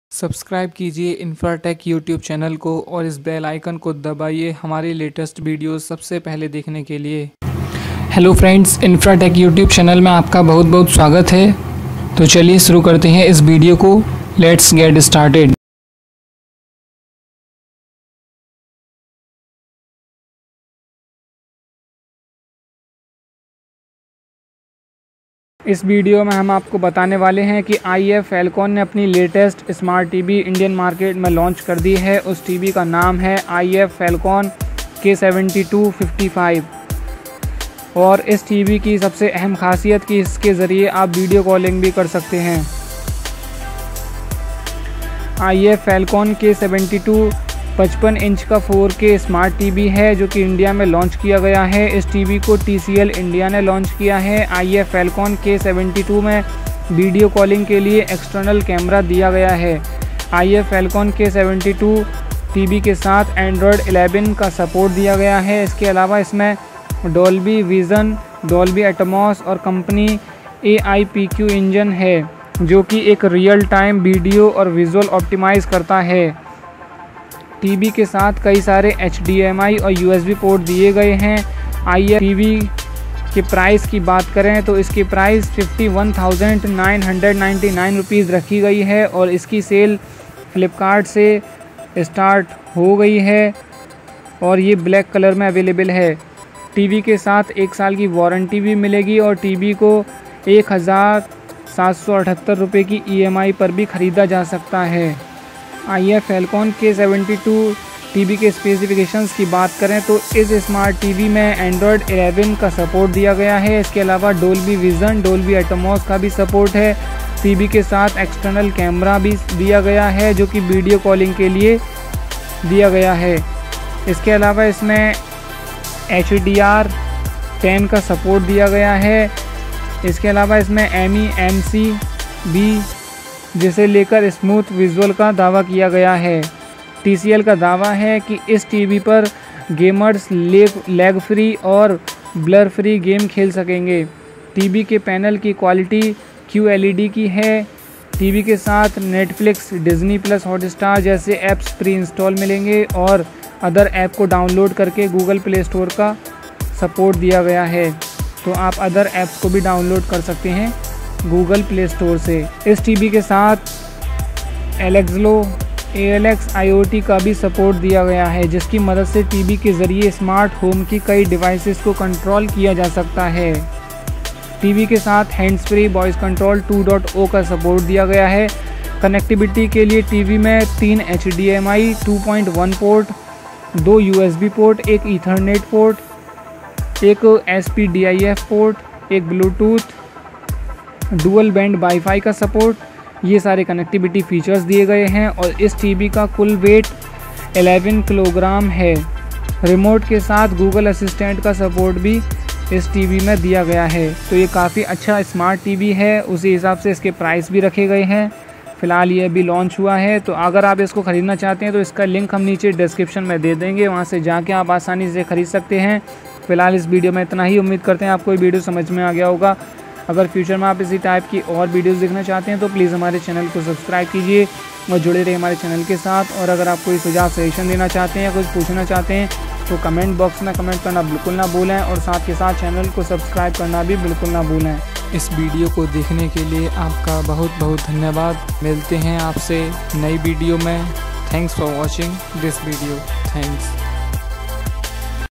Hindi !! iF Falcon Smart TV launched Price, Specifications & Review | Infra Tech | TCL
In this video I make a review of iF Falcon Smart TV launched in India.